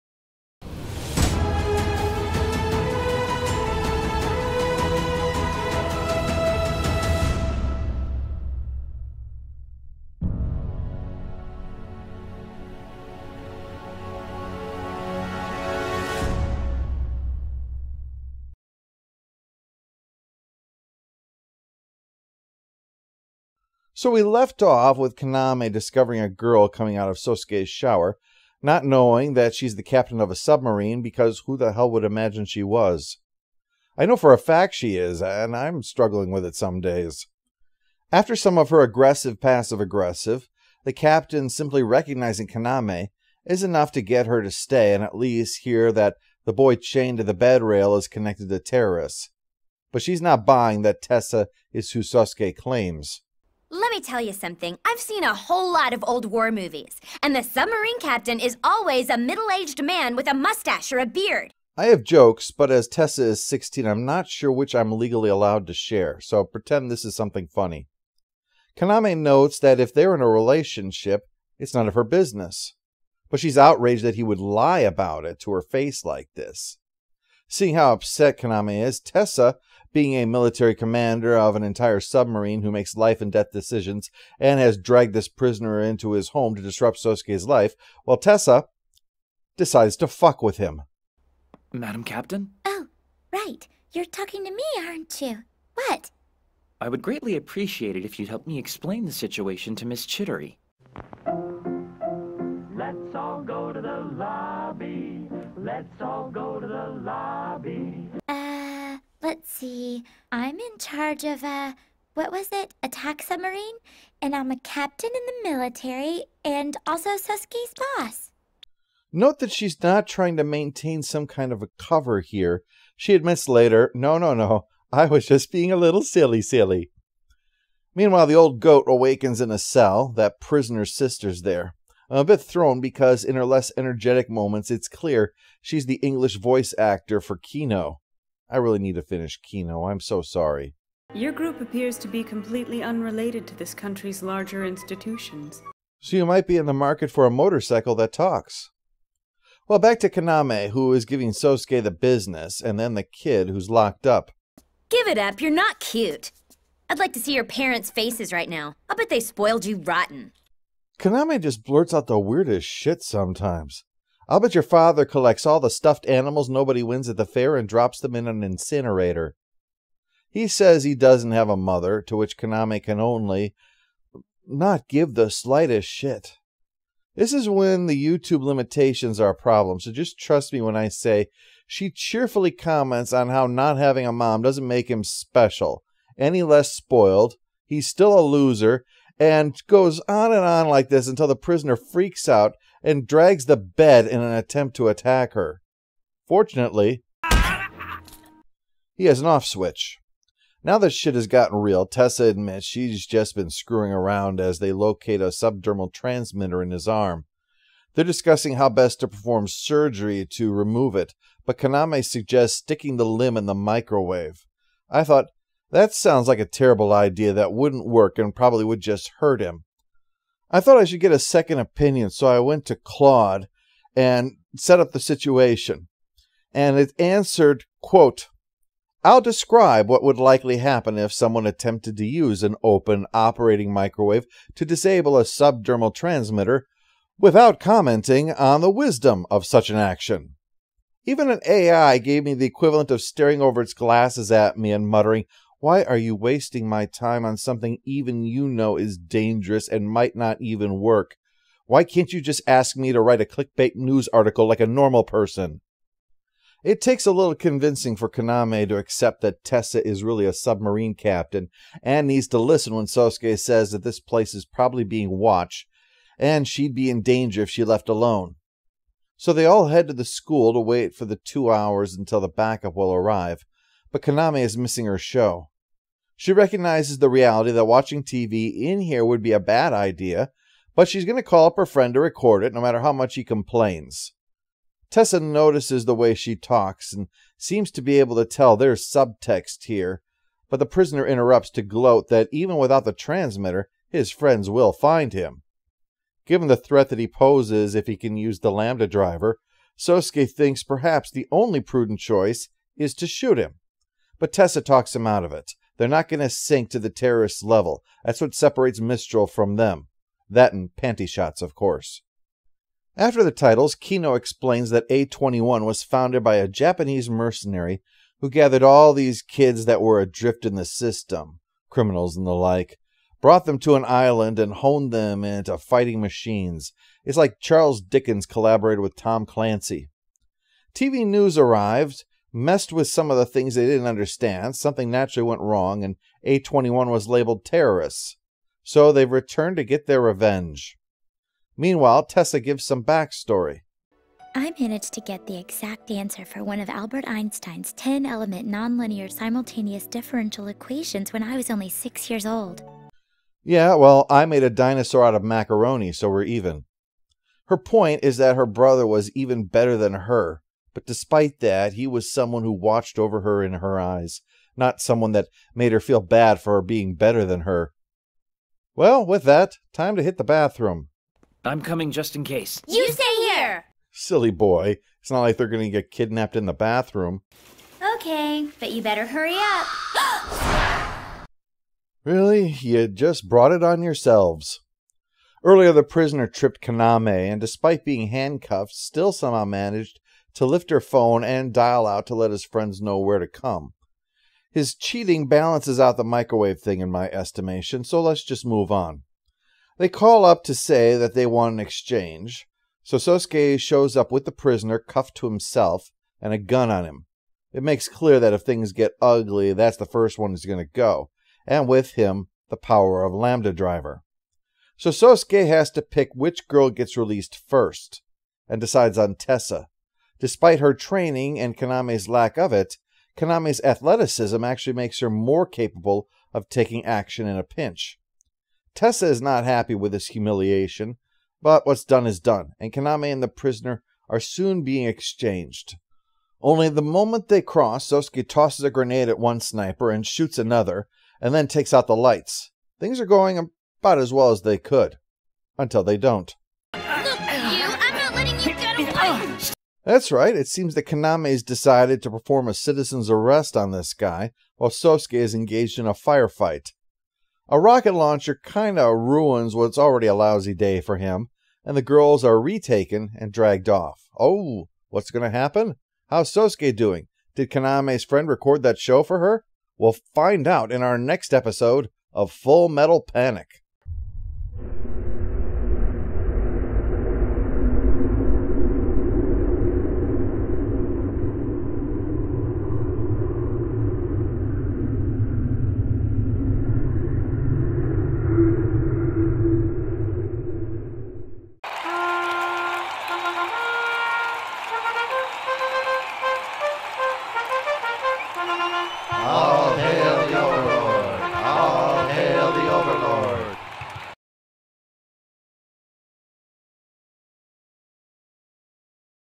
A Look at Full Metal Panic Episode 10
The romantic interest gets dragged into things, involving unsafe appliance use and why you should always have an escort to pee.